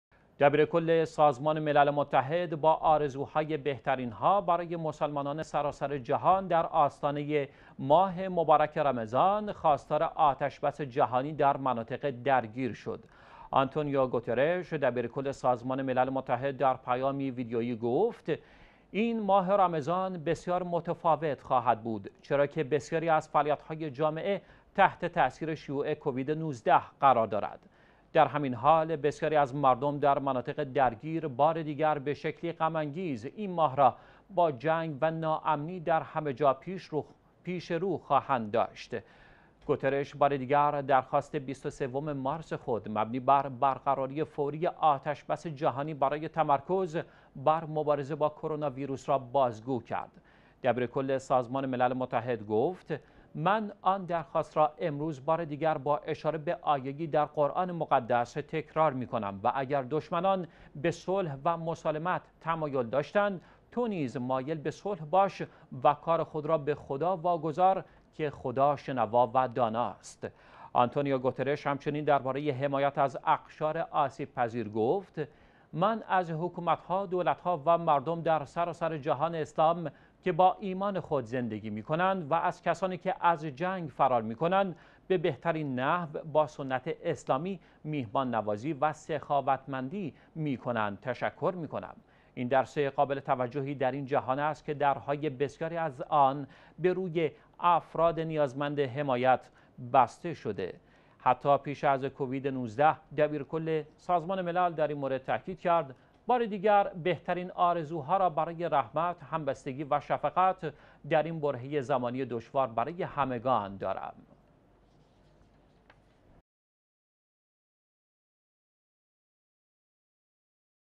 دبیر کل سازمان ملل در پیامی به مناسبت ماه رمضان خواستار آتش‌بس جهانی شد
دبیرکل سازمان ملل متحد با آرزوهای بهترین‌ها برای مسلمانان سراسر جهان در آستانه ماه مبارک رمضان خواستار آتش‌بس جهانی در مناطق درگیر شد.
آنتونیو گوترش، دبیرکل سازمان ملل متحد در پیامی ویدیویی گفت: «این ماه رمضان بسیار متفاوت خواهد بود، چرا که بسیاری از فعالیت‌های جامعه تحت تاثیر شیوع کووید-۱۹ قرار دارد. در همین حال، بسیاری از مردم در مناطق درگیر بار دیگر به شکلی غم‌انگیز این ماه را با جنگ و ناامنی در همه جا پیش رو خواهند داشت.»
 گوترش بار دیگر درخواست ۲۳ مارس خود مبنی بر برقراری فوری آتش‌بس جهانی برای تمرکز بر مبارزه با کروناویروس را بازگو کرد.
دبیرکل سازمان ملل متحد گفت: «من آن درخواست را امروز بار دیگر با اشاره‌ به آیه‌ای در قرآن مقدس تکرار می‌کنم؛ و اگر دشمنان به صلح و مسالمت تمایل داشتند تو نیز مایل به صلح باش و کار خود به خدا واگذار که خدا شنوا و داناست.»
آنتونیو گوترش همچنین درباره حمایت از اقشار آسیب‌پذیر گفت: «من از حکومت ها، دولت ها و مردم در سراسر جهان اسلام که با ایمان خود زندگی می‌کنند و از کسانی که از جنگ فرار می‌کنند به بهترین نحو با سنت اسلامی میهمان‌نوازی و سخاوتمندی می‌کنند، تشکر می‌کنم. این درس قابل توجهی در این جهان است که درهای بسیاری در آن به روی افراد نیازمند حمایت بسته شده، حتی پیش از کووید-۱۹.»
دبیرکل سازمان ملل متحد تاکید کرد: بار دیگر بهترین آرزوها را برای رحمت، همبستگی و شفقت در این برهه زمانی دشوار برای همگان دارم.